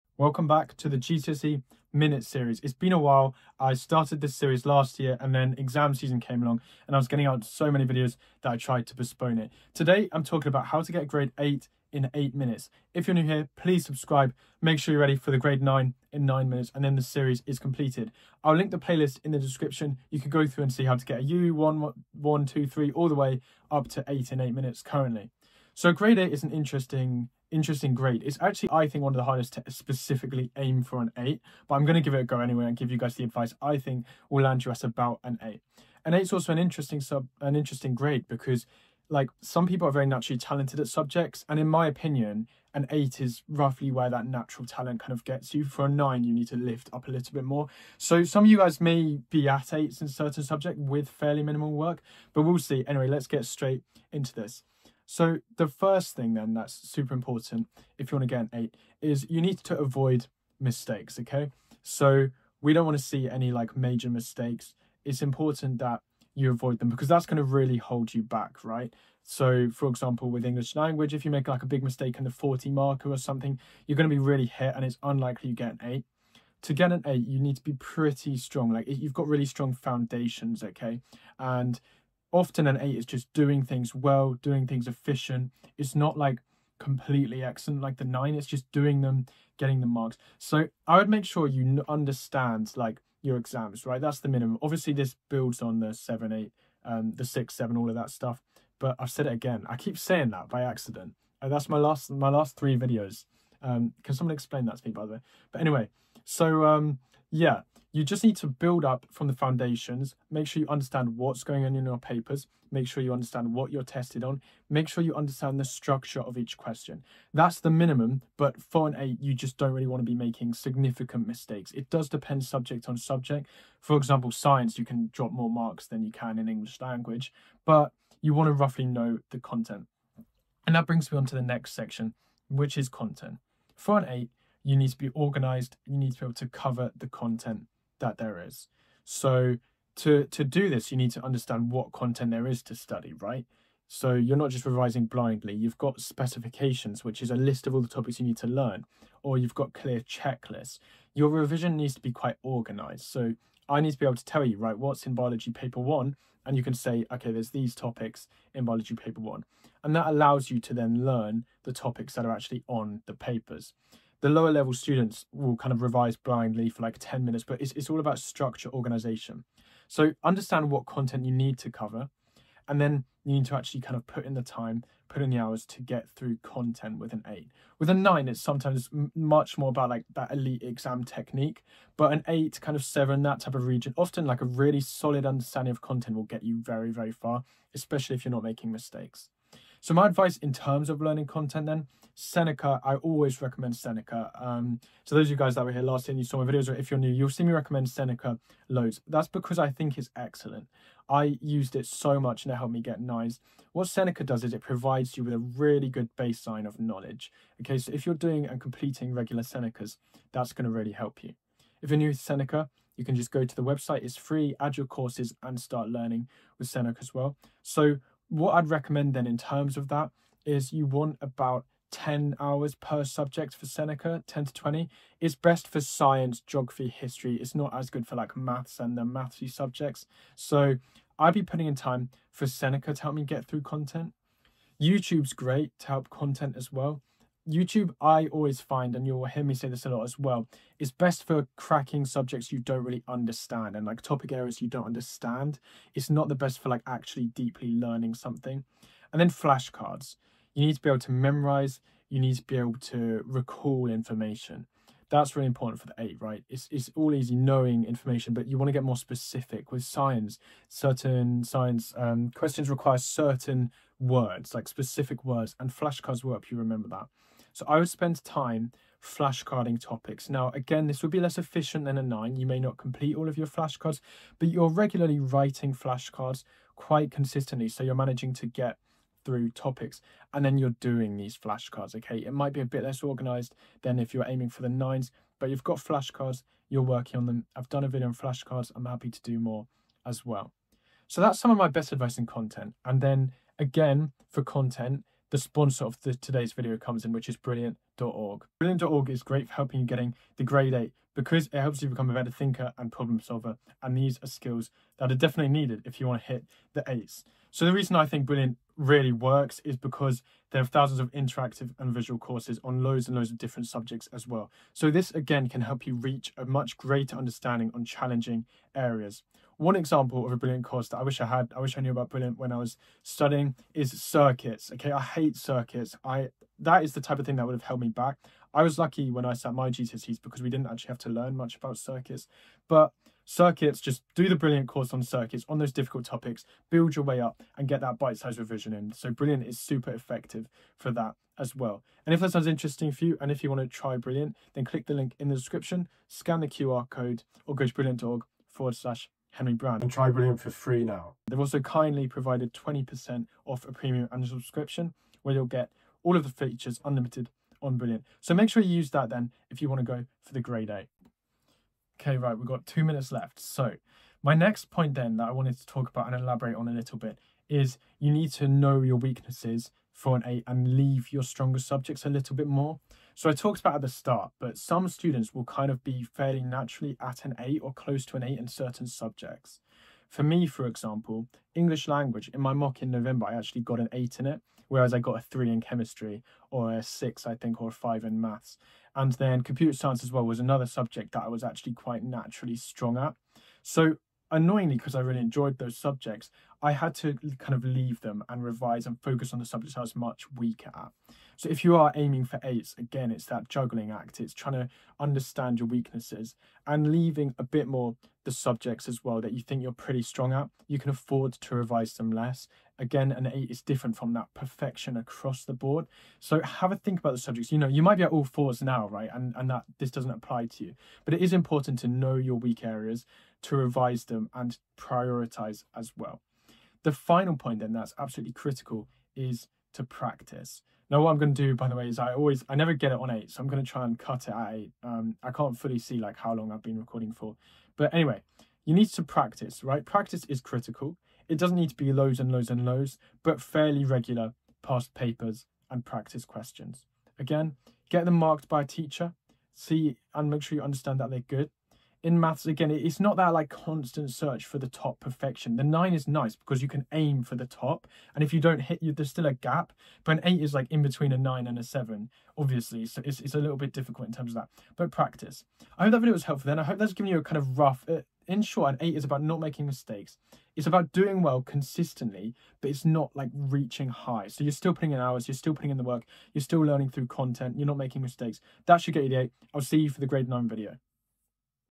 How to Get a GCSE Grade 8 in 8 Minutes!
⌛TIMESTAMPS:
00:00 Introduction
00:28 A grade 8
01:00 Avoiding mistakes
02:12 Content
06:07 Prioritisation
07:09 Practice
07:50 Conclusion